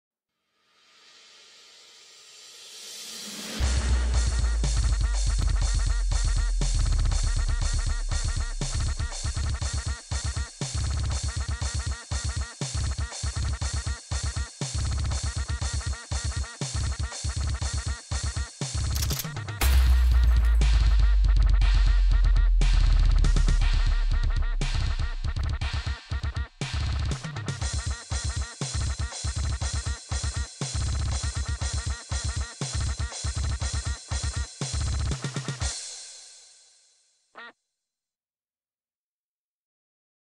The Future of Djent
Some label djent as an unoriginal genre that anyone can just sit down and play, but this new style of djent could prove differently. I believe this to be the next big genre in the metal scene that'll never die out.